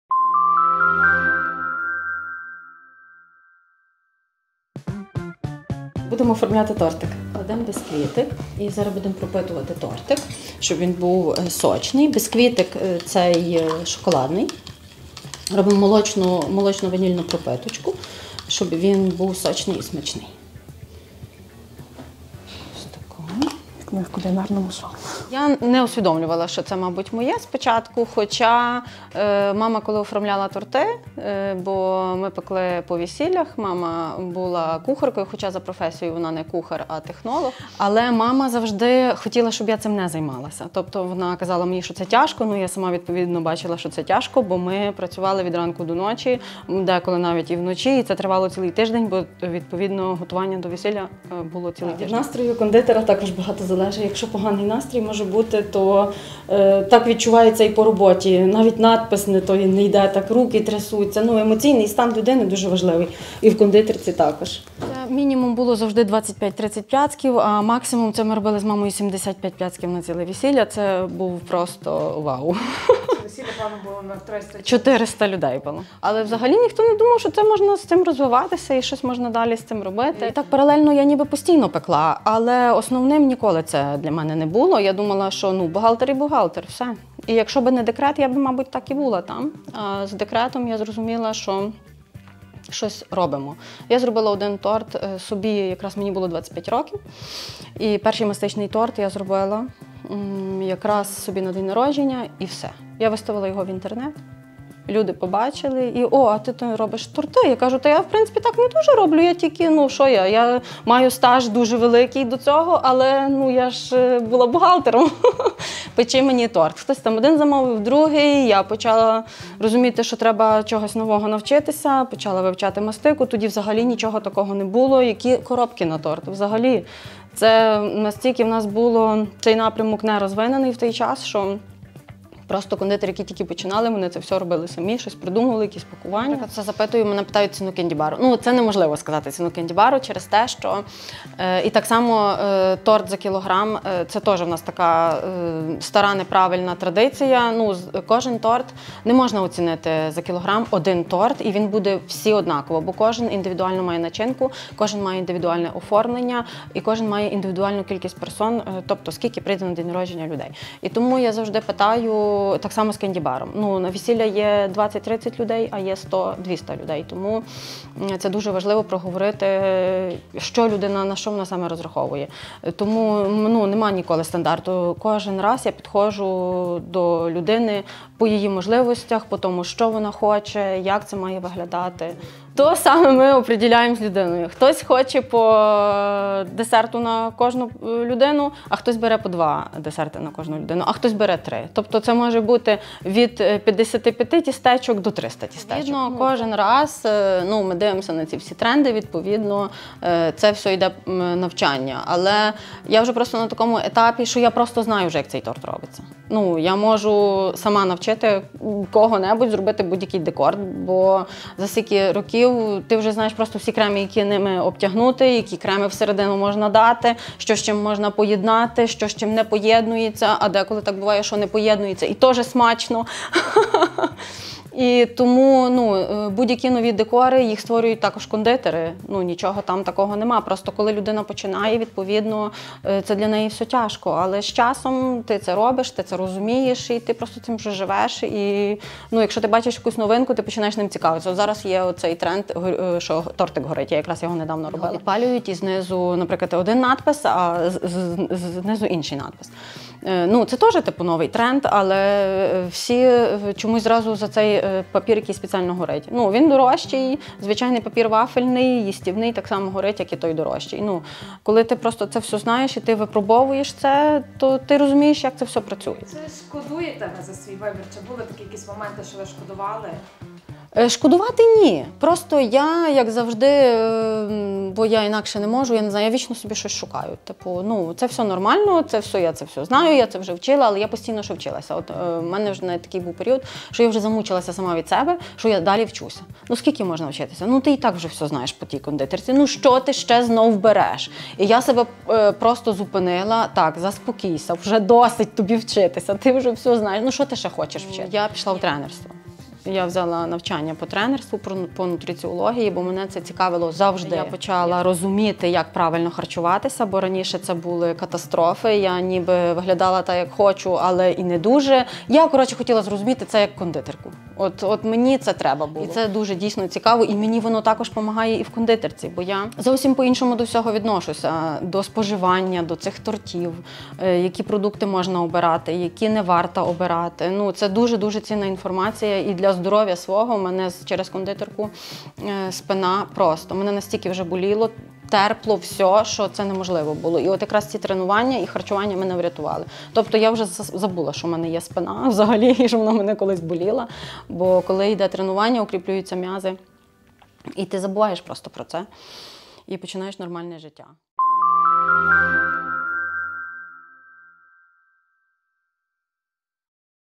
"Спартак" та "П’яна вишня" для мене залишаються фаворитами — кондитерка Марічка Каназирська
Урізноманітнює своїми солодощами, тістечками, макарунами, зефірними букетами, десертами, пляцками та тортами будь-яке свято, адже уже більше двадцяти років у цій сфері. За плечима безліч навчань і самовдосконалень, аналізів та пошуків — і це не тільки в кондитерці. Якою є Марічка Каназирська поза роботою та що важливе у професійній діяльності, розповіла та навіть частково продемонструвала «Вікнам».